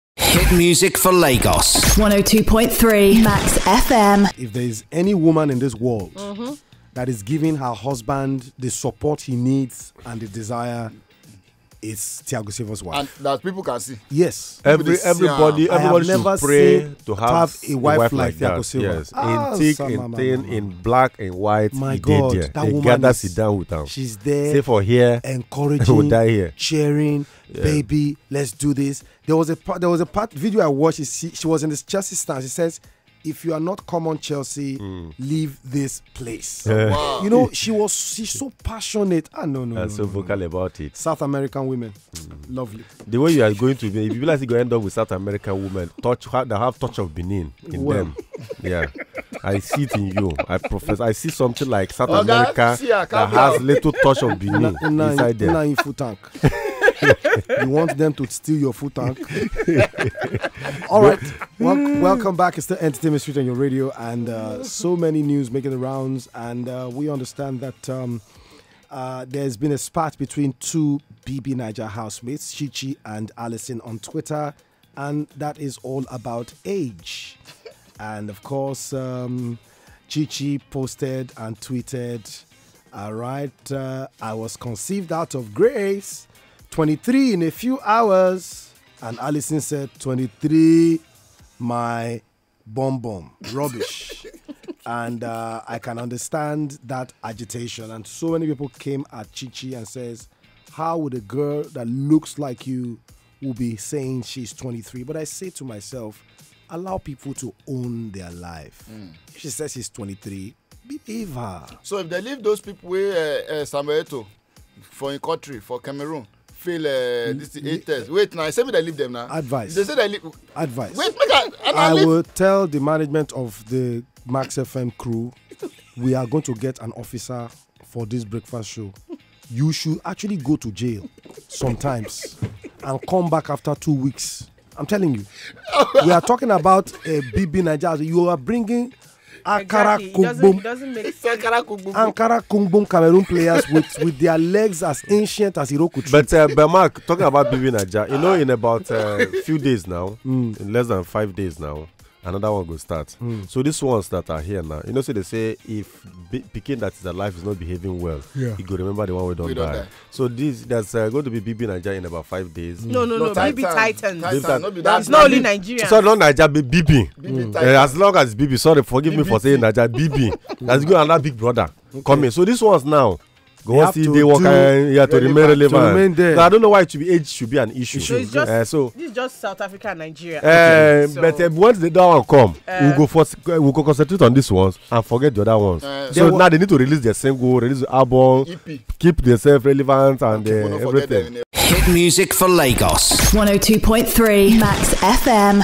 BBN Allyson Calls Out Colleague ChiChi For Allegedly Lying Over Age [SEE VIDEO]
Core entertainment updates